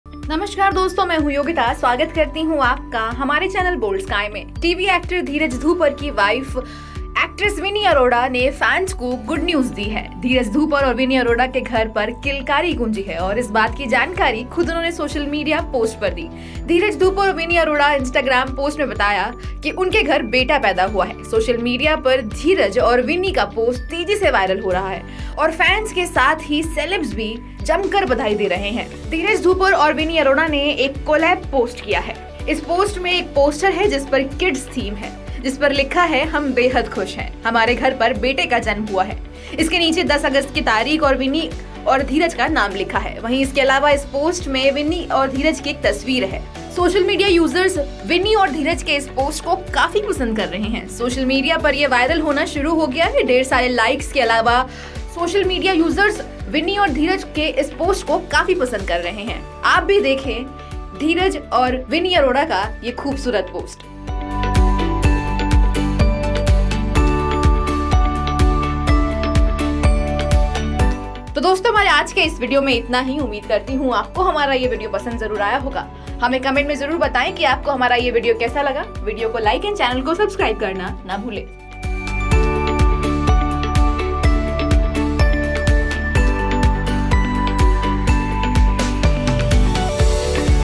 Dheeraj Dhoopar बने Father Wife Vinny Arora ने दिया Baby Boy को जन्म | Boldsky*Entertainment
TV actor Dheeraj Dhoopar's wife and actress Vinny Arora has given good news to the fans. Kilkari has echoed at Dheeraj Dhoopar and Vinny Arora's house and he himself has given this information on his social media post. Dheeraj Dhoopar and Vinny Arora have told in an Instagram post that a son has been born in their house. Dheeraj and Vinny's post on social media is becoming increasingly viral and fans as well as celebs are congratulating them.

टीवी एक्टर धीरज धूपर (Dheeraj Dhoopar) की वाइफ व एक्ट्रेस विन्नी अरोड़ा (Vinny Arora) ने फैन्स को गुड न्यूज दी है। धीरज धूपर और विन्नी अरोड़ा के घर पर किलकारी गूंजी है और इस बात की जानकारी खुद उन्होंने सोशल मीडिया पोस्ट पर दी है। धीरज धूपर और विन्नी अरोड़ा इंस्टाग्राम पोस्ट में बताया है कि उनके घर में बेटा पैदा हुआ है। सोशल मीडिया पर धीरज और विन्नी का पोस्ट तेजी से वायरल हो रहा है और फैन्स के साथ ही सेलेब्स भी बधाई दे रहे हैं।